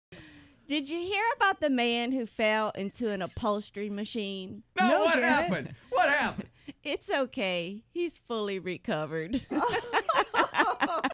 The Upholstery Machine - Janet's Joke, January 25, 2017
Did you hear about the man who fell into an upholstery machine?